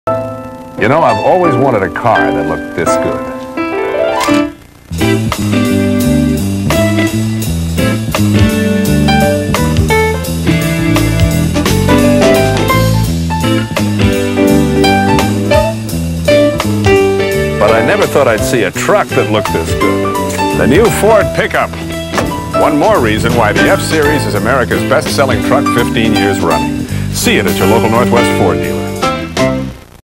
Ford F Series - Truck Pickup Commercial (1992)
Transferred from a VHS recording of NBC on February 23, 1992. During an airing of the TV Movie What She Doesn't Know